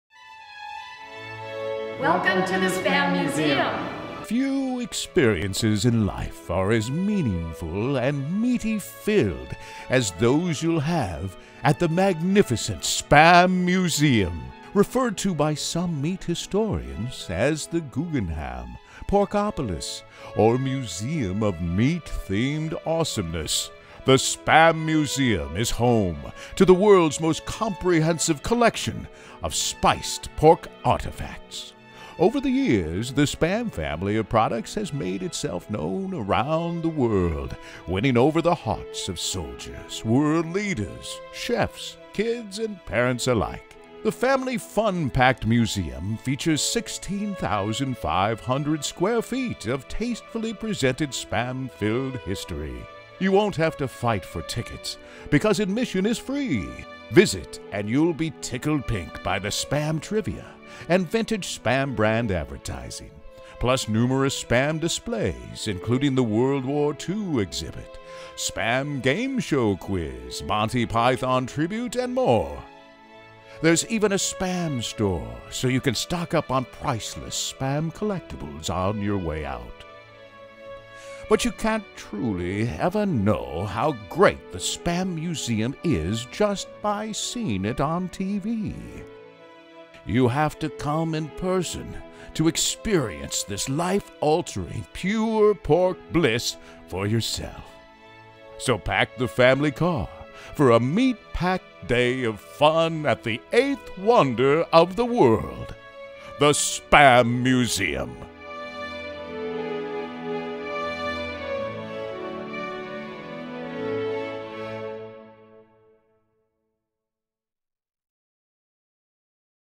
THE SPAM MUSEUM -- BEST THEME MUSEUM -- MINNESOTA 2014
Few experiences in life are as meaningful and meaty-filled as those you'll have at the magnificent SPAM Museum. Referred to by some meat historians as The Guggenham, Porkopolis or Museum Of Meat-Themed Awesomeness, the SPAM Museum is home to the world's most comprehensive collection of spiced pork artifacts.